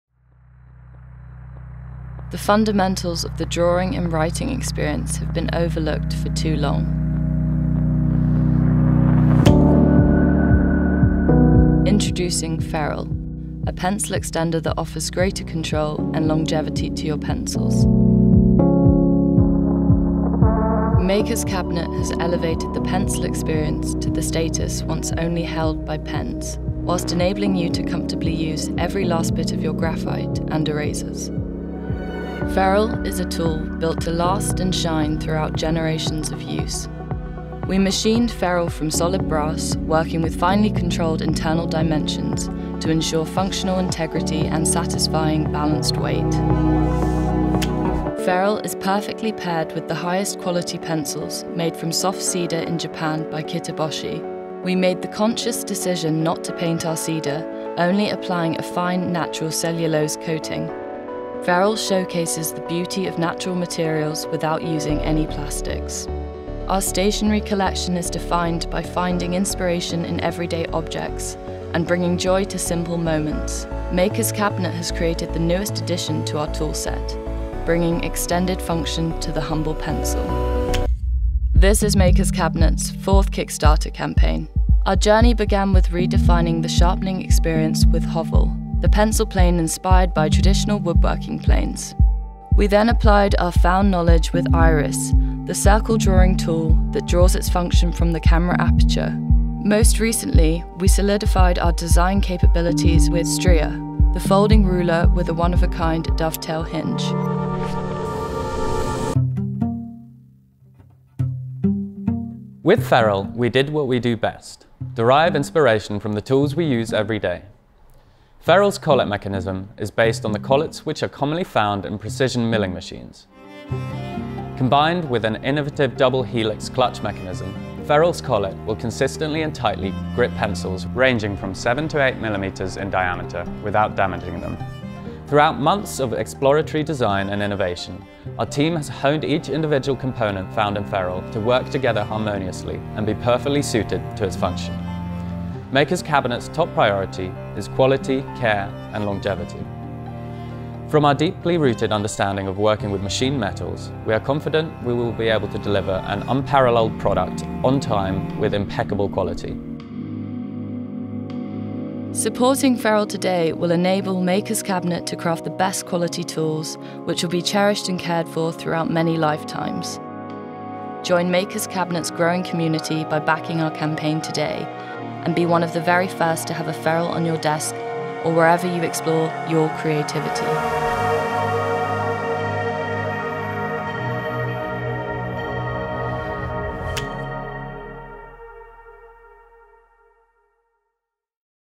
Ferrule, a pencil holder that will extend the life of your pencils and last generations.
Ferrule will be live on Kickstarter from April 13th for a limited time.

Ferrule is a pencil holder which simultaneously takes the best features of pencil extenders, the most cherishable attributes of premium pen design, and the thoughtfulness of mechanical pencils to create a statement work of art that will minimise wastage throughout your work.

Back our campaign to one of the first to receive a Ferrule and help Makers Cabinet continue redesigning the tools we use every day.